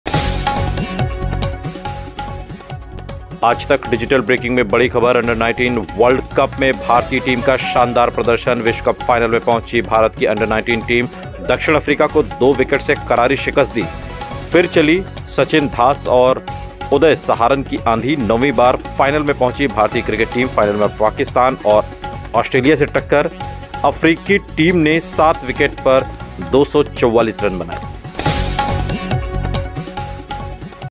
BREAKING NEWS: 9वीं बार U-19 World Cup Final में पहुंची भारतीय टीम | Cricket Aaj Tak News
साउथ अफ्रीका की मेजबानी में खेले जा रहे अंडर-19 वर्ल्ड कप 2024 में भारतीय टीम ने धांसू प्रदर्शन करते हुए फाइनल में एंट्री कर ली है. उसने सेमीफाइनल मुकाबले में मेजबान अफ्रीका को ही 2 विकेट से करारी शिकस्त दी. भारतीय टीम 9वीं बार फाइनल में पहुंची है.